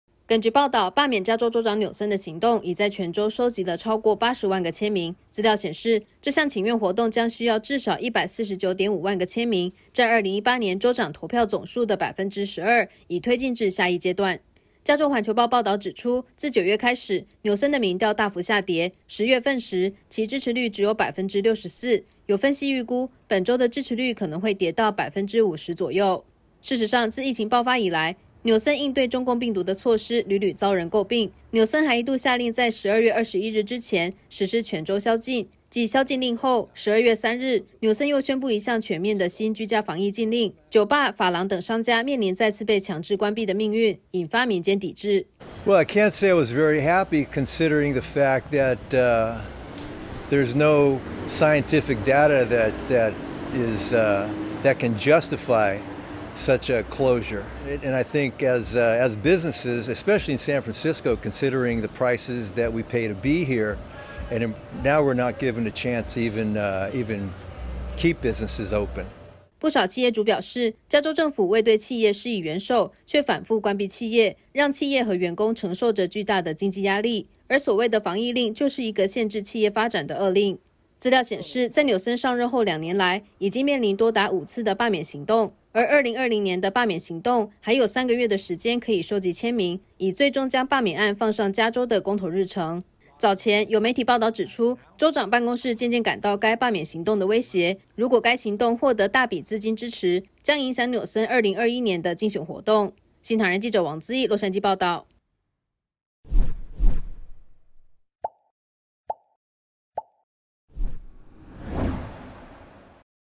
罷免紐森門檻150萬人 民間已獲過半簽名
疫情爆發以來，加州民眾面對州長紐森的施政作為，促使罷免紐森的活動遍地開花。截止週四（10日），該連署活動已經獲得超過80萬個簽名，距離150萬罷免門檻剩下不到一半。